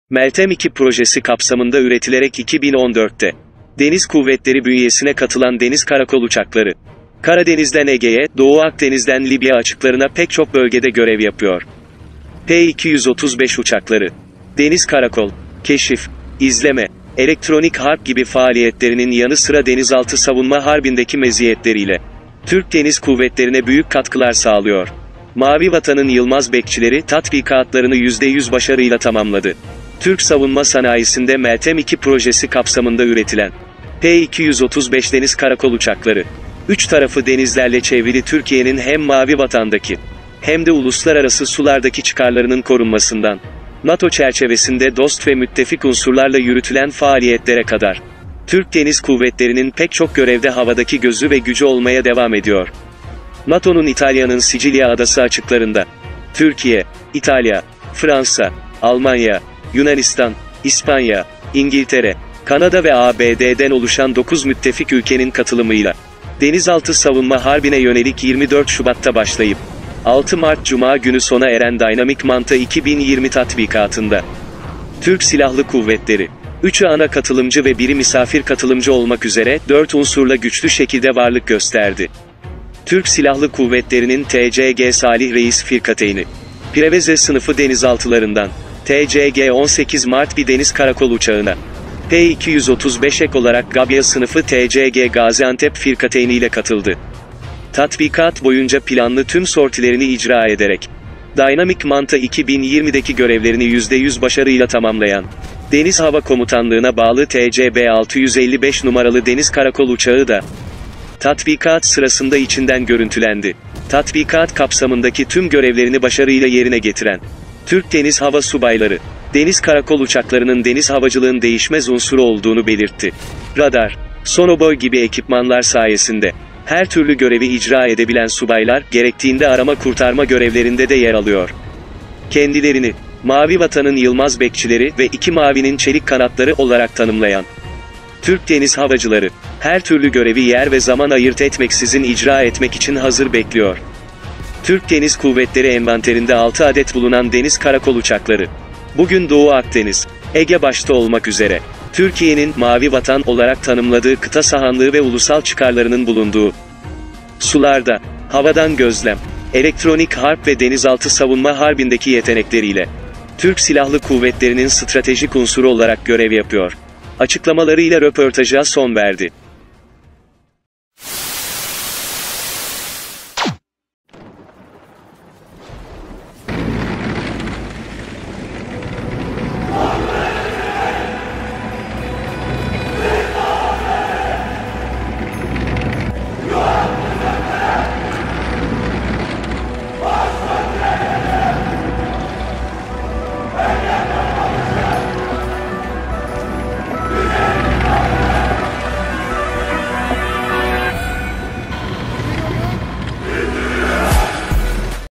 Türk Donanmasının Havadaki Gözü: P235 Deniz Karakol Uçağı
17 yardımcı sınıf gemi, 14 karakol gemisi, 22 çıkarma gemisi, 1 eğitim gemisi olmak üzere toplam 103 gemiyle, ayrıca deniz karakol uçakları, deniz helikopterleri, insansız hava araçları, SAT, SAS görev timleri, amfibi deniz piyade taburları, Kara Kuvvetleri Komutanlığı'ndan genel maksat ve taarruz helikopterleri, Hava Kuvvetleri Komutanlığı'ndan taktik hava gayreti ve havadan ihbar kontrol uçağı, Sahil Güvenlik Komutanlığından ise Sahil Güvenlik gemi ve botlarıyla iştirak etti.